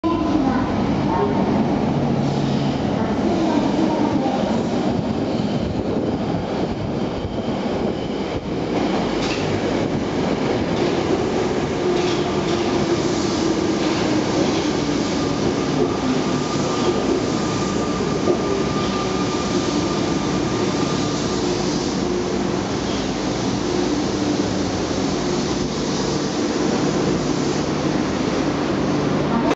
東京メトロ01系第28編成銀座線 溜池山王駅到着
東京メトロ01系第28編成銀座線の溜池山王駅到着シーンです。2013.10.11撮影